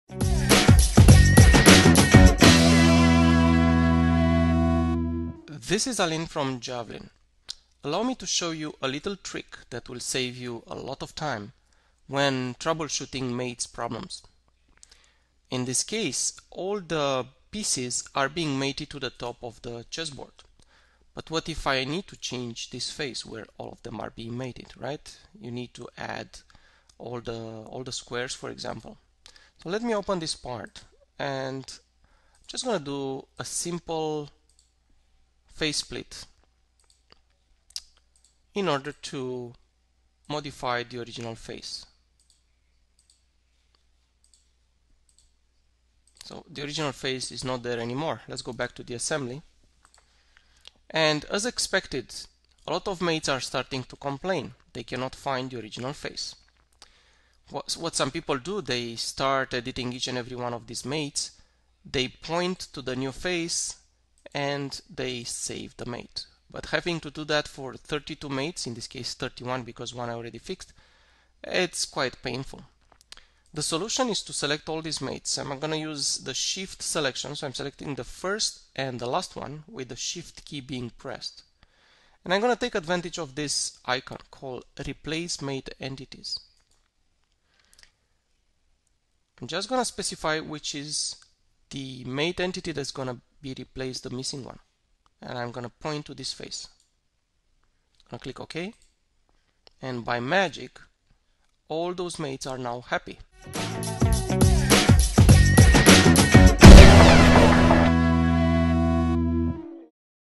Replace Mate Entities in Bulk in SolidWorks Assemblies
When entities referred in existing mates (faces, edges, vertices, planes) change, the result is often a long list of errors in your assembly feature tree.

This video presents an elegant way to replace all the defective mate references in bulk in order to save a lot of time.